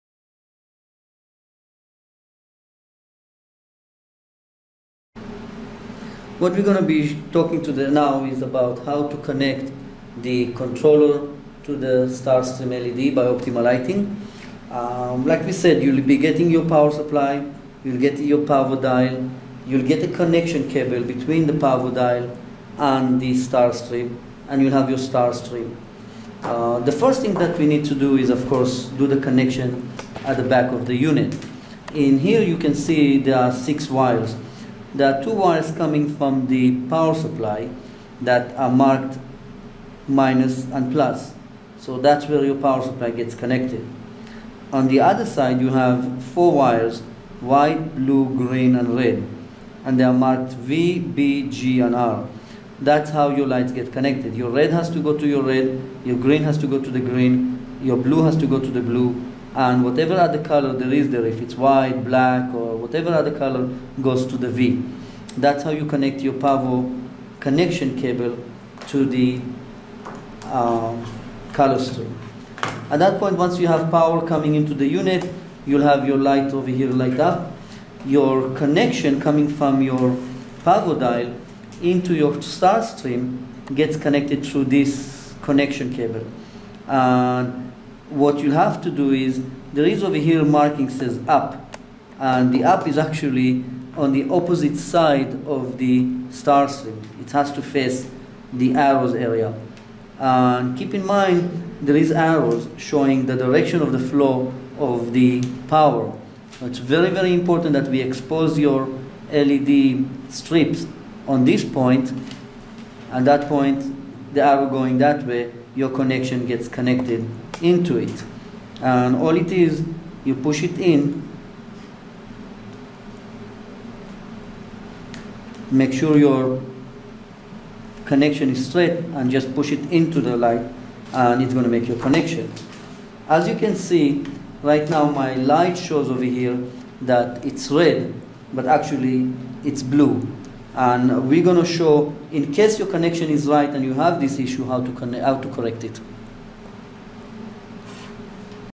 COLORStream - Conecting the PAVO-DIAL
how to connect the PAVO-DIAL RGB LED Controller to COLORStream RGB LED Strip